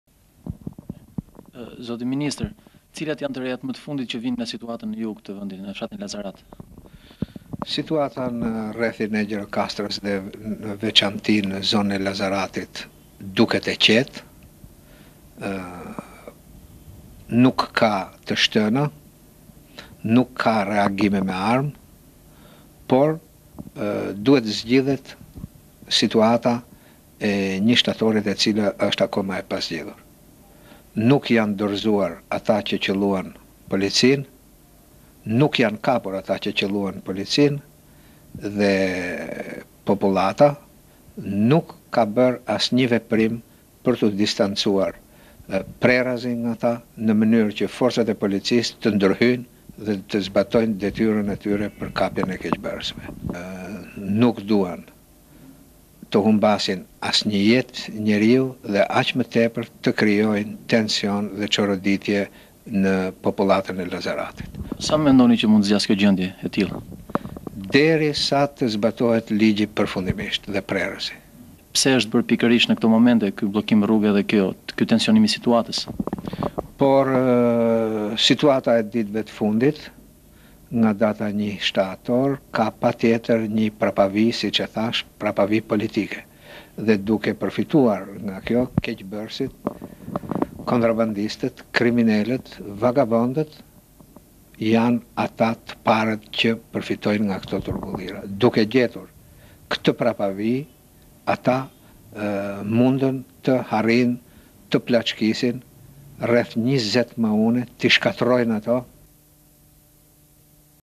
Lajme Arkiva - Perikli Teta per situaten ne Lazarat (04 Shtator 1998)
Arkiva - Tv Klan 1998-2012; Pamje ministri i Rendit Perikli Teta interviste.

Ministri i Rendit Perikli Teta  flet në intervistë për situatën në Lazarat. Sipas ministrit Teta situata në Lazarat është e qetë pas ngjarjeve të fundit ku ndodhen plaçkitje dhe bllokim të rrugës Kakavijë-Gjirokastër, por dhe popullata aty duhet të jetë në bashkëpunim të policisë.

"Situata në rrethin e Gjirokastrës dhe në veçanti në Lazarat duket e qete, nuk ka të shtëna me armë por nuk janë dorëzuar dhe nuk janë kapur ata që qëlluan policinë. Popullata nuk ka bërë asnjë veprim për tu distancuar nga këta kriminelë në mënyre që forcat e policisë të ndërhyjnë për kapjen e tyre. Kjo gjendje ka për të zgjatur derisa të zbatohet ligji përfundimisht. Situata ka një prapavijë politike dhe duke përfituar nga kjo,keqbërësit kriminelë janë të parët që përfitojnë nga kjo situatë."- u shpreh ai

Gazetar : Roland Zili